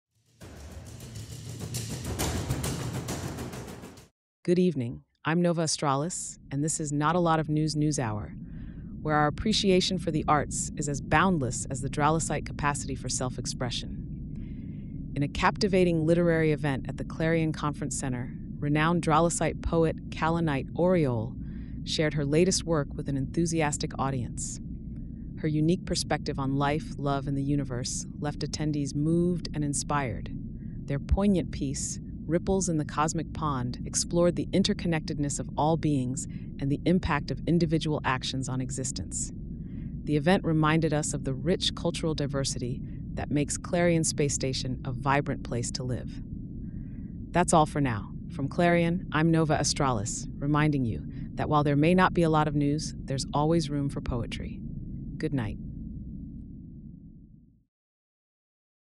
Cast 23 - Dralasite poet, Kalanit~Aureole,
In this heartwarming segment of Not-A-Lot-Of-News News Hour, Nova Astralis reports on a captivating literary event held at the Clarion Cultural Center. Renowned Dralasite poet, Kalanit~Aureole, takes center stage, sharing their latest work with an enthusiastic audience. Kalanit~Aureole's unique perspective on life, love, and the universe leaves attendees moved and inspired, particularly with their poignant piece, "Ripples in the Cosmic Pond," which explores the interconnectedness of all beings and the impact of individual actions on existence. Through Kalanit~Aureole's eloquent use of metaphor and fluid delivery style, viewers are treated to a showcase of the beauty and depth of Dralasite art. This event serves as a reminder of the rich cultural diversity that makes Clarion Space Station such a vibrant place to live. Tune in to experience the profound verses of Kalanit~Aureole and celebrate the power of poetry in bringing together the community on Clarion.